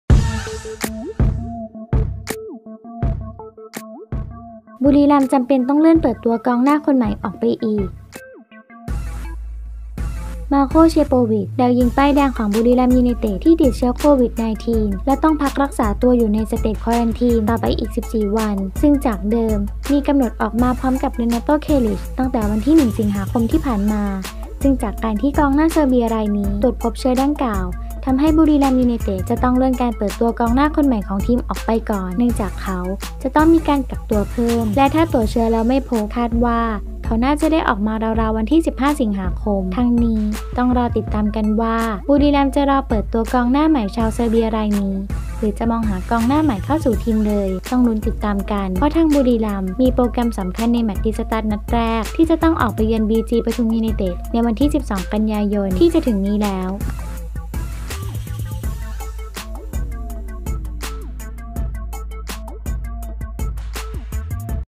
"บุรีรัมย์" จำเป็นต้องเลื่อนเปิดตัวกองหน้าคนใหม่ออกไปอีก
มาร์โค เชโปวิช ดาวยิงป้ายแดงของ บุรีรัมย์ ยูไนเต็ด ที่ติดเชื้อโควิด-19 และต้องพักรักษาตัวอยู่ใน State Quarantine ต่อไปอีก 14 วัน ซึ่งจากเดิมมีกำหนดออกมาพร้อมกับ เรนาโต้ เคลิช ตั้งแต่วันที่ 1 สิงหาคมที่ผ่านมา

ซึ่งจากการที่กองหน้าเซอร์เบียรายนี้ตรวจพบเชื้อดังกล่าว ทำให้ บุรีรัมย์ ยูไนเต็ด จะต้องเลื่อนการเปิดตัวกองหน้าคนใหม่ของทีมออกไปก่อน เนื่องจากเขาจะต้องมีการกักตัวเพิ่ม และถ้าตรวจเชื้อแล้วไม่พบคาดว่าเขาน่าจะได้ออกมาราวๆ วันที่ 15 สิงหาคม